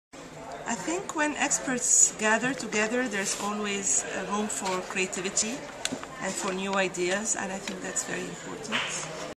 Global IGF 2009: Hala Tadros
Hala Tadros, Egypt; Cyberpeace Initiative, talks about whether the IGF mandate should be extended beyond the five-year term.